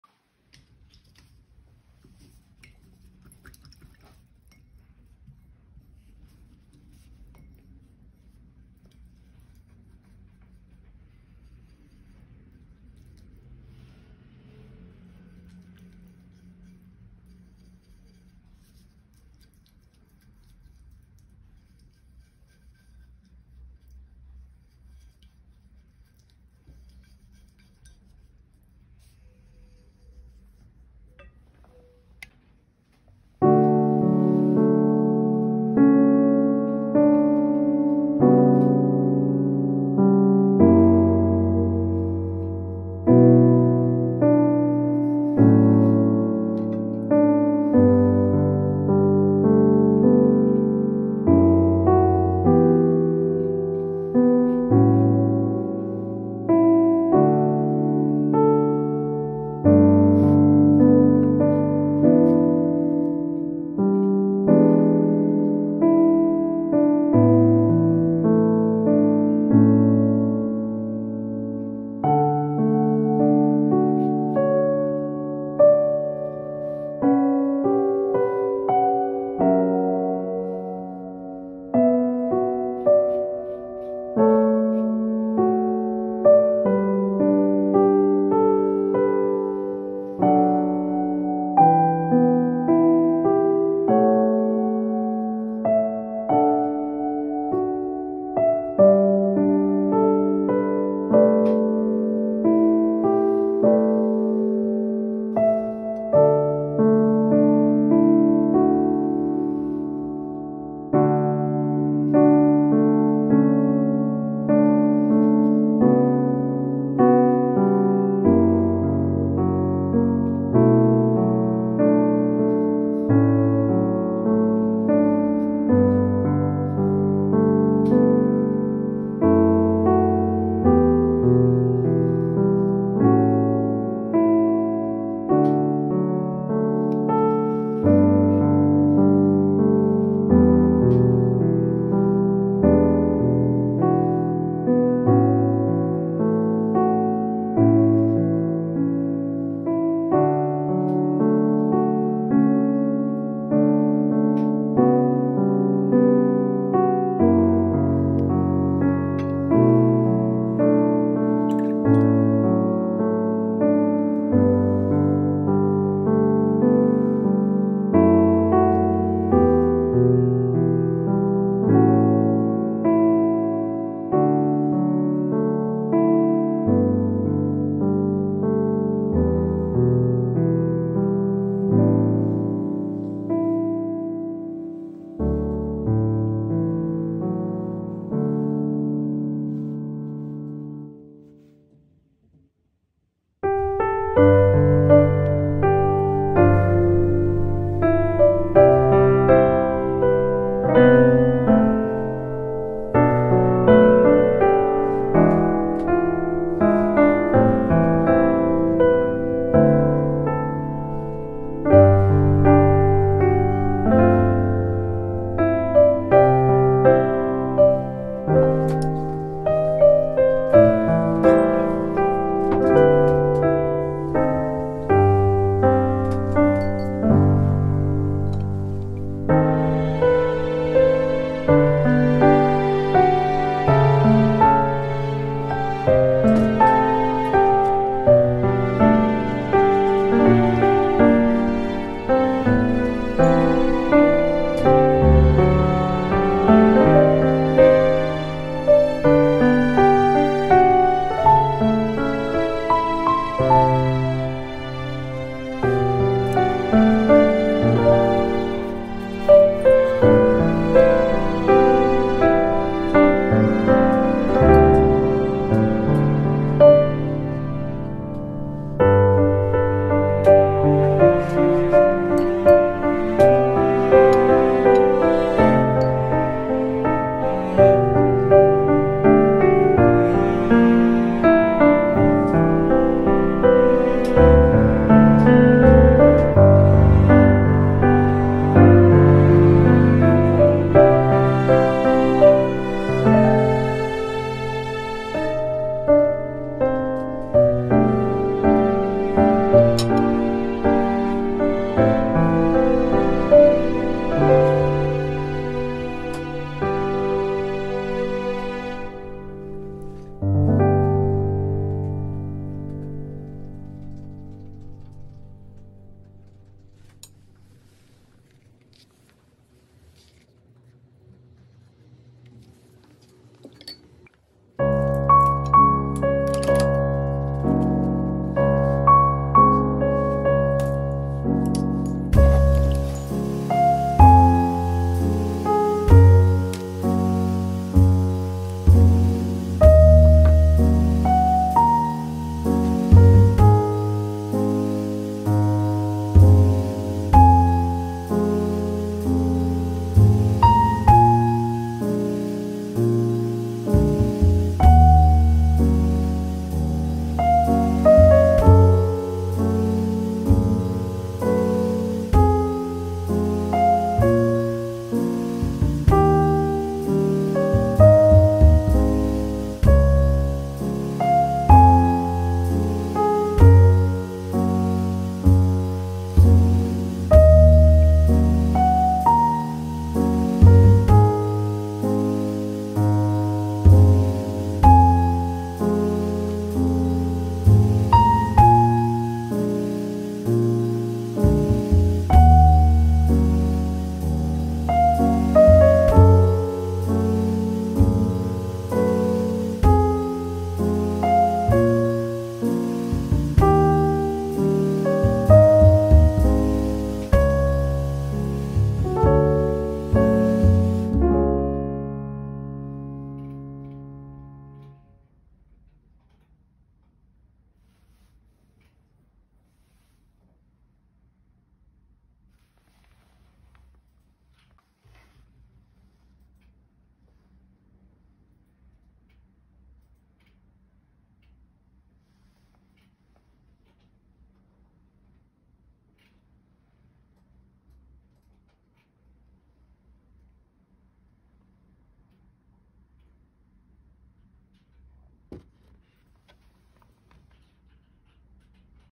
수채화로 꽃 그리기 _ 붓꽃 _ Watercolor flower painting_ Iris
안녕하세요, 수채화로 붓꽃을 그려봤어요.
물감과 물감이 겹쳐져서 레이어가 여러겹이 되어 깊이감이 생깁니다.
허나 겹치더라도 탁해지지 않도록 !
붓꽃의 꽃말은  좋은 소식 ! 이라고 하네요.
모두에게 좋은 소식이 있길~
나에게 좋은 소식은 뭘까, 내일  맑은 하늘이 있길 기대해봅니다.